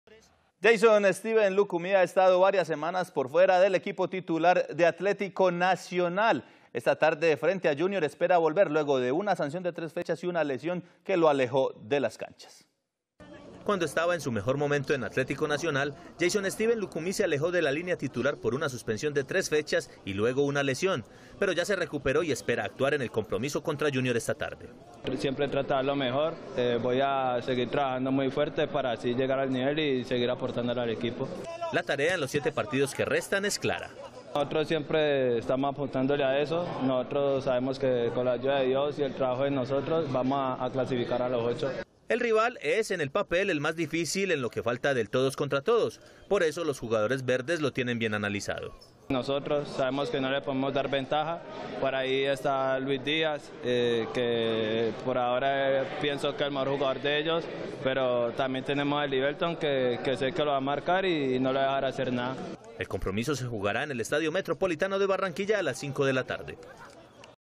Jeison Lucumí espera ser titular con Nacional ante Junior [Noticia] - Telemedellín
Lucumí se alejó de la línea titular por una suspensión de tres fechas y luego una lesión, ahora espera actuar en el compromiso contra Junior esta tarde.